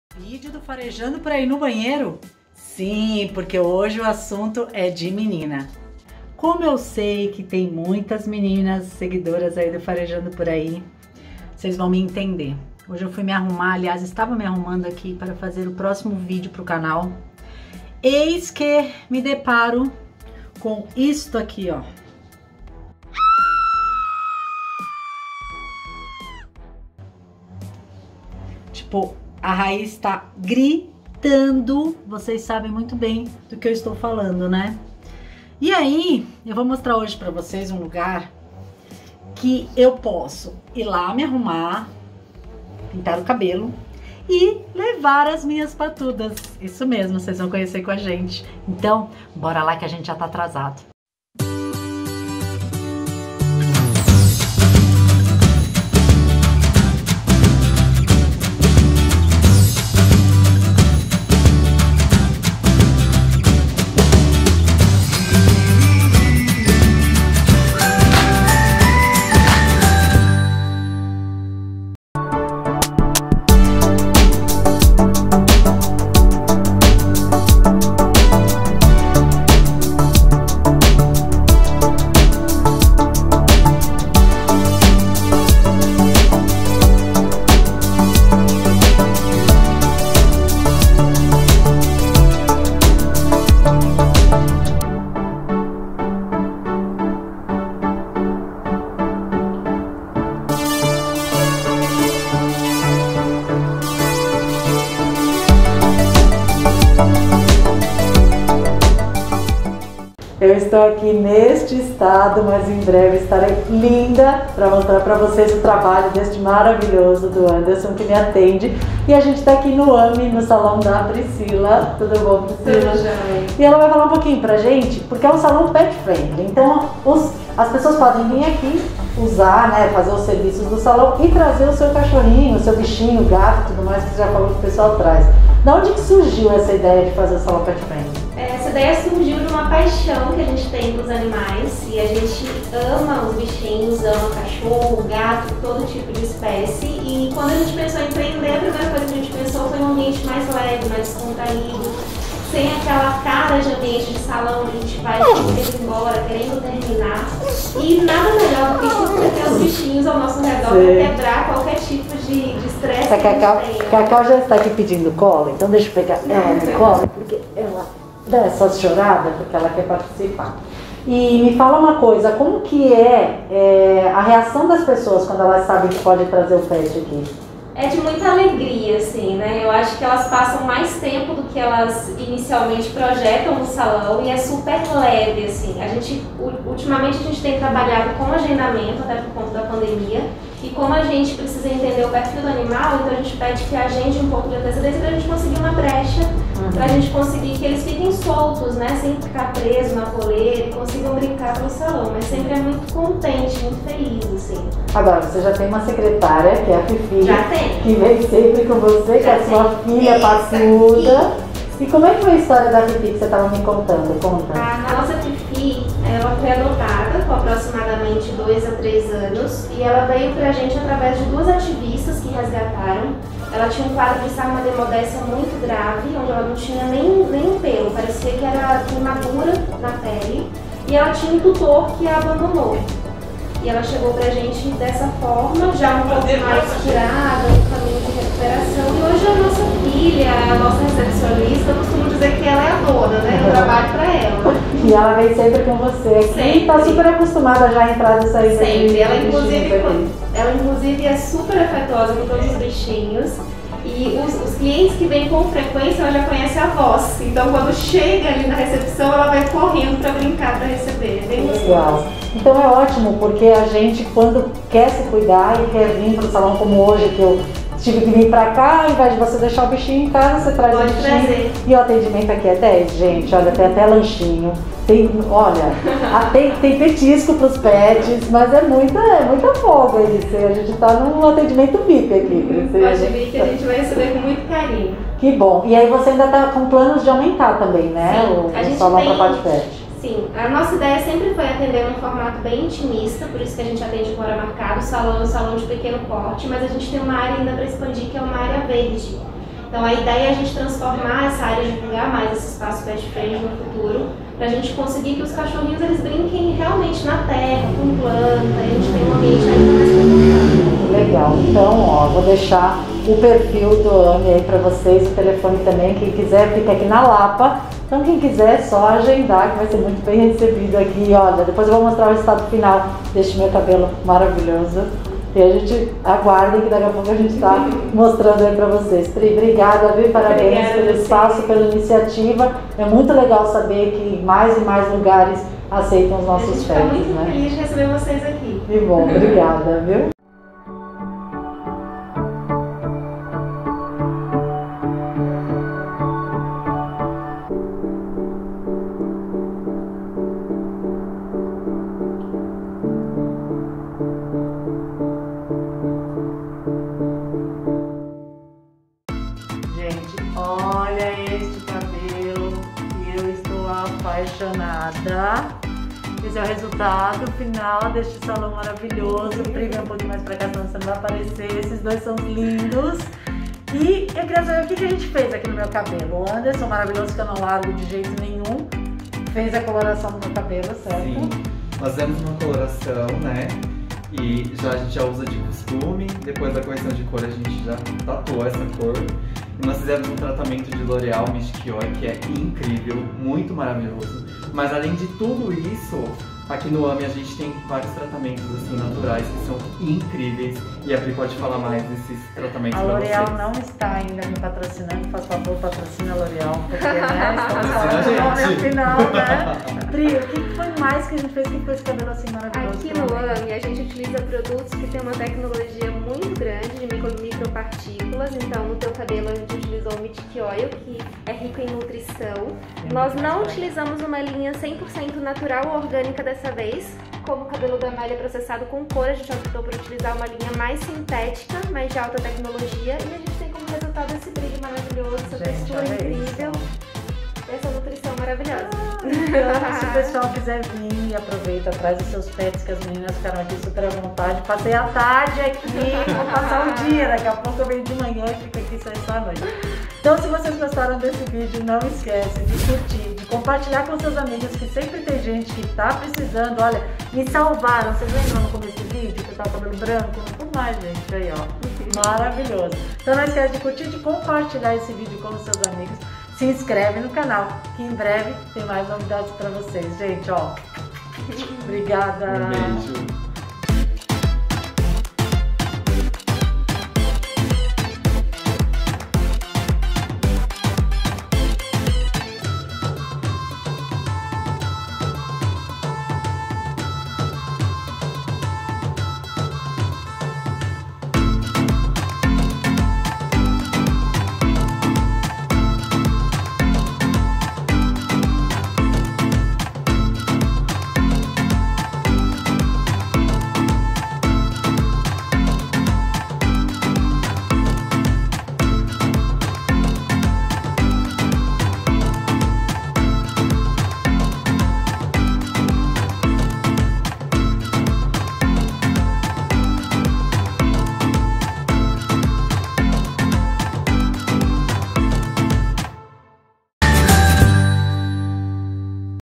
Salão de beleza que aceita cachorro
Fomos farejar no Salão de beleza Ame, aqui em São Paulo. Pensa em um atendimento VIP com direito a lanchinho e petiscos. Eu adorei essa ideia, poder me cuidar e levar minhas patudas junto comigo.
E você já conhecia um salão assim? tem alguma salão assim aí na sua região?
Conta para gente aqui nos comentários.